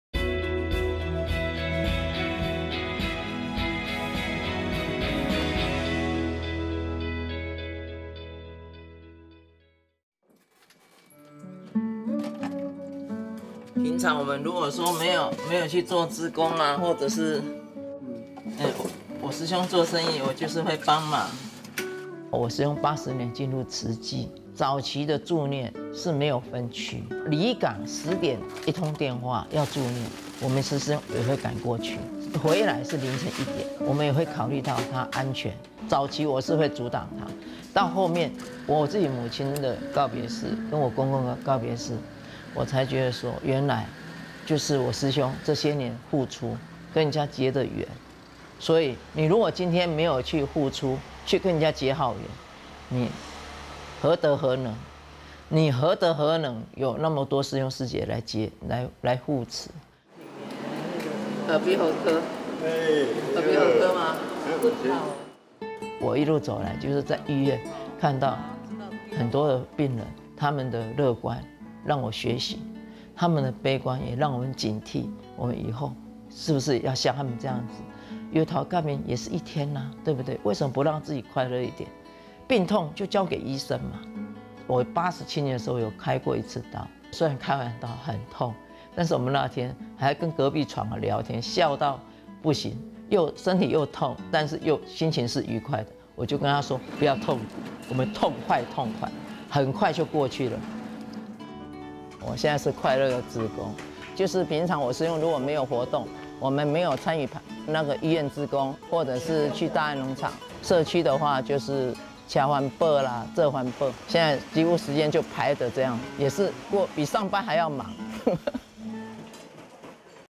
Living Bodhisattvas - Chiu Kuei-chu (人間菩薩 - 邱桂珠)
Seeing the suffering of those who are ill while volunteering at Dalin Tzu Chi Hospital in Taiwan, Chiu Kuei-chu has learned to count her blessings and look at the bright side of things. Let's see her story!

在慈濟醫院當志工的經驗，讓邱桂珠學到寶貴的一課，懂得用更加樂觀的態度來面對一切。一起來看看她的故事！

Tzu Chi Culture and Communication Foundation